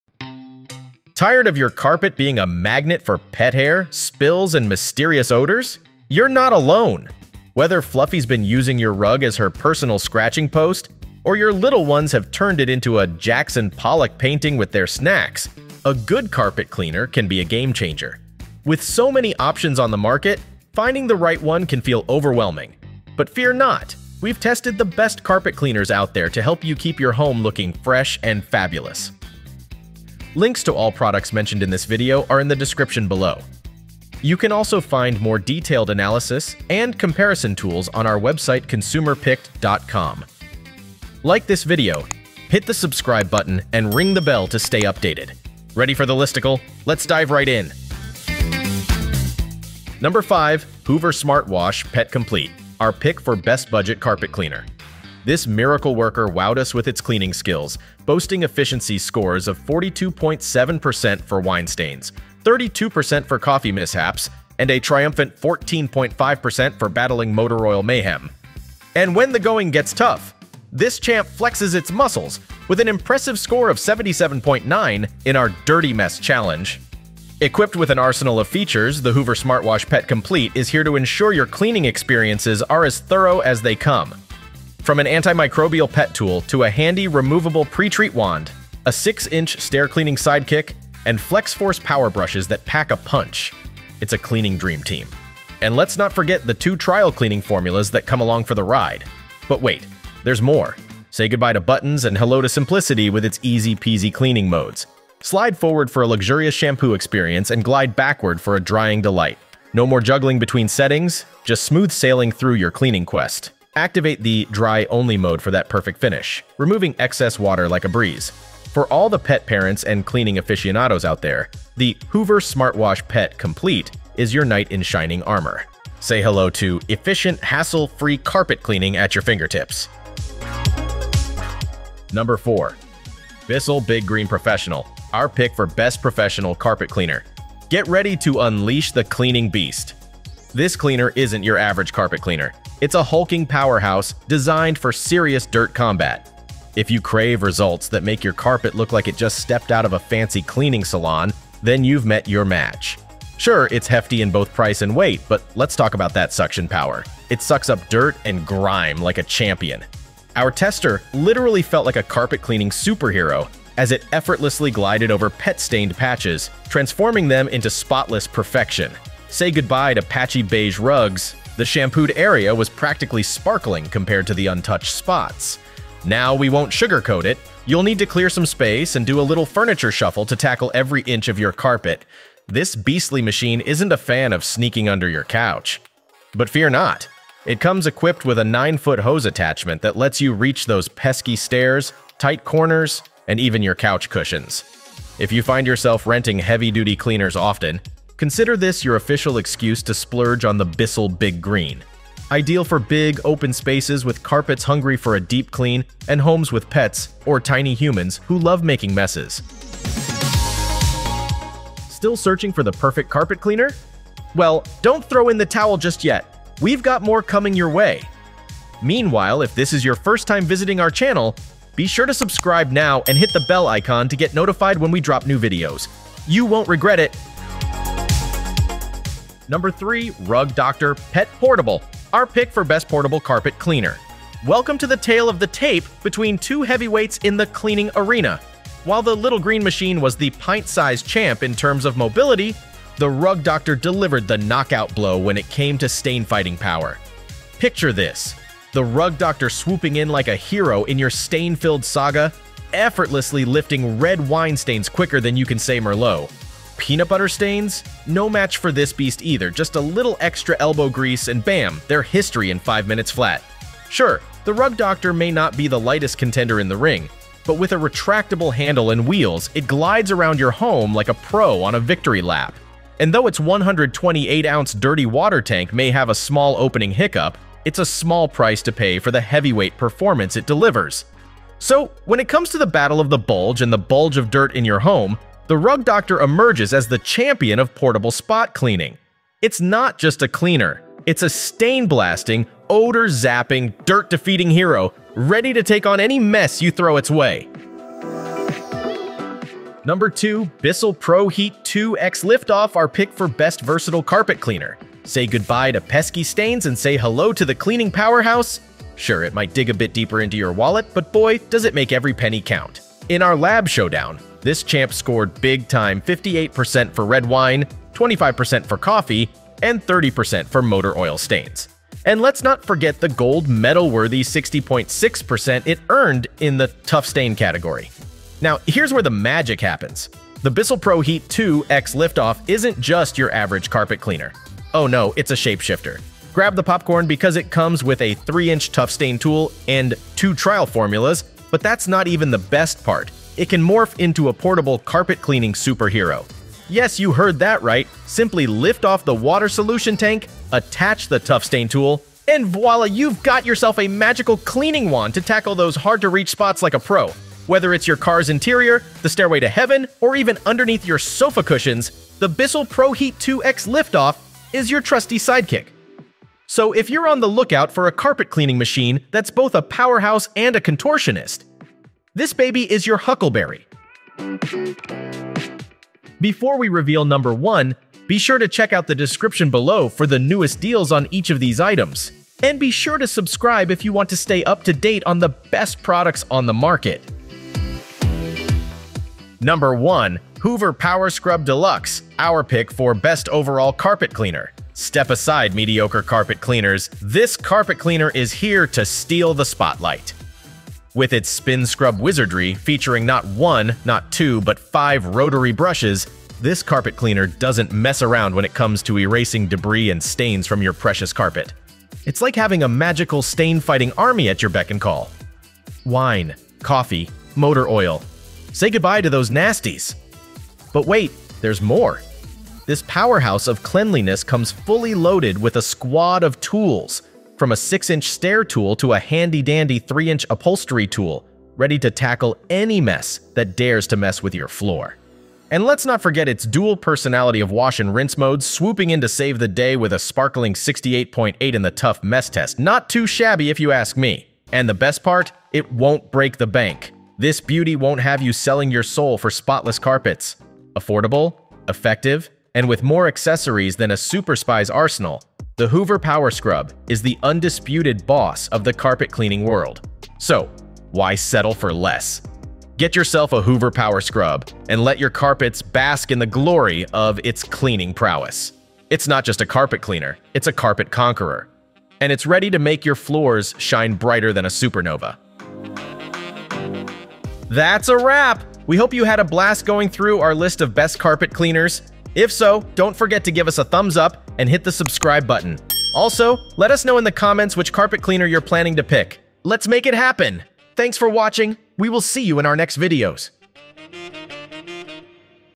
Best Carpet Cleaners 2024 - (Which One Is The Best?)
➡ Links of the Best Carpet Cleaners we listed in this video.

1. Hoover PowerScrub Deluxe – Best Overall Carpet Cleaner

2. Bissell ProHeat 2X Lift-Off – Best Versatile Carpet Cleaner

3. Rug Doctor Pet Portable – Best Portable Carpet Cleaner

4. Bissell Big Green Professional – Best Professional Carpet Cleaner

5. Hoover Smartwash Pet Complete - Best Budget Carpet Cleaner

Discover the ultimate solutions for maintaining spotless carpets with our top picks for the best carpet cleaners. Leading the way is the Hoover PowerScrub Deluxe, acclaimed as the Best Overall Carpet Cleaner for its powerful performance and ease of use. The Bissell ProHeat 2X Lift-Off shines as the Best Versatile Carpet Cleaner, offering a detachable canister for spot cleaning. For those needing a compact option, the Rug Doctor Pet Portable stands out as the Best Portable Carpet Cleaner, perfect for tackling pet stains and small messes. The Bissell Big Green Professional, hailed as the Best Professional Carpet Cleaner, delivers exceptional deep cleaning capabilities, rivaling professional services. Lastly, the Hoover Smartwash Pet Complete earns its place as the Best Budget Carpet Cleaner, providing great value without compromising on performance. Explore these options to find the perfect carpet cleaner that meets your needs and keeps your home looking pristine.